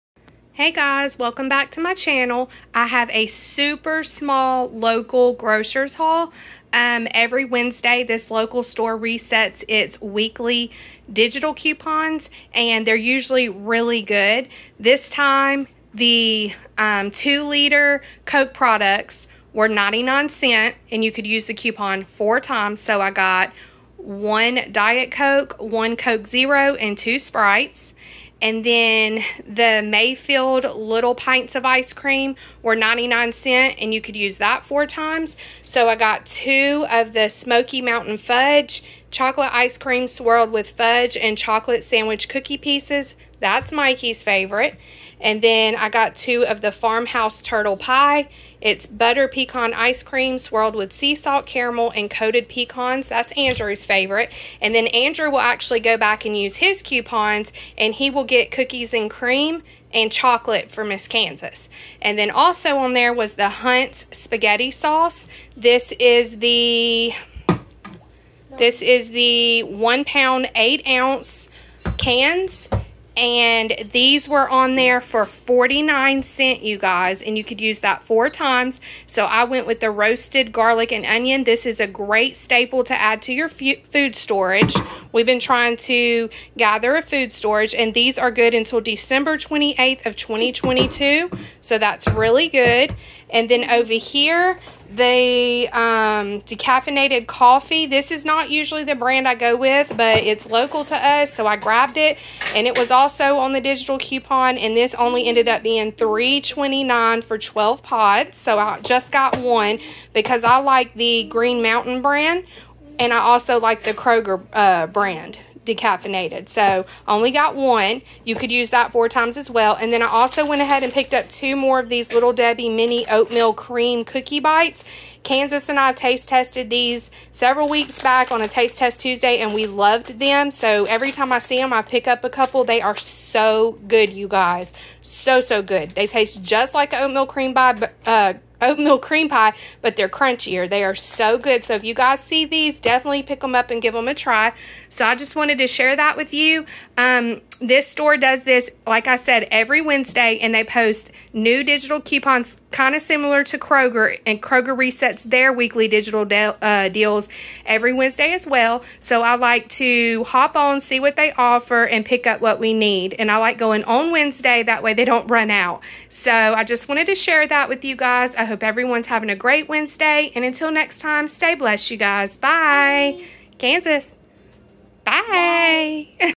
Small Local Grocery Haul! 9-8-21
Hey you guys!!! 🌻🌻🌻 I have a Small Local Grocery Haul for y'all! Just a few items from the weekly digital deals. Pantry Stockup!

All items were purchased with my own money. All opinions are my own.